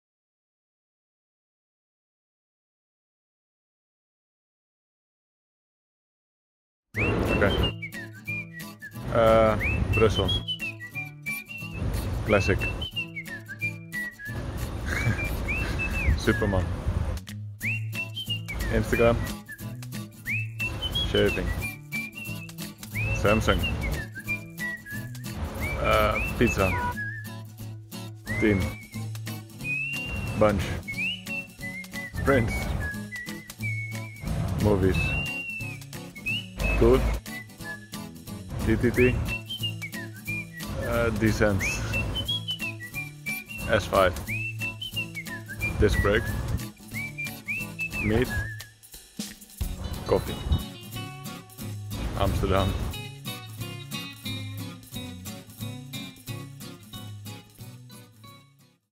Get to Know | Cees Bol
Get to know one of our TDF2019🇫🇷 debutants, Cees Bol, a bit better with some quick fire questions🔥🔥🔥